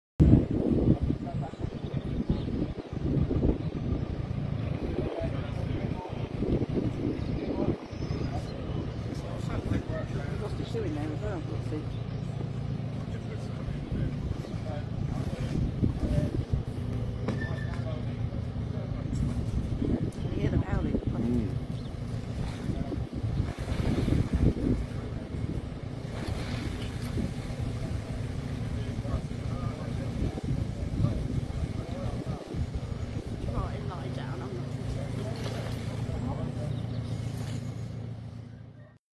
Horse rescued from swimming pool by fire service in Leicestershire
The owner of a horse called the emergency services when it fell into a pool, which had a cover on it, and couldn't get out.

A crew from Oakham fire station attended the scene along with a specialist animal rescue team and a vet to hoist the animal to safety.